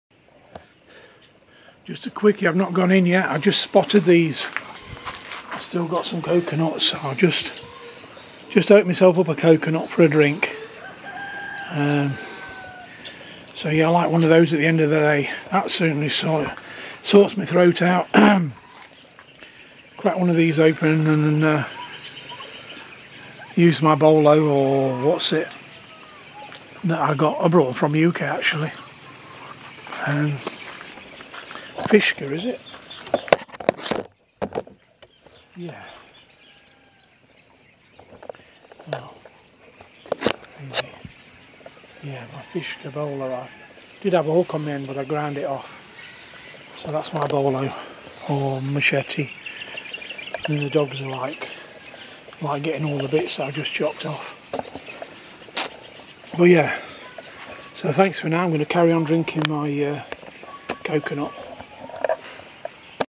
coconut
its nice when you can harvest from your own trees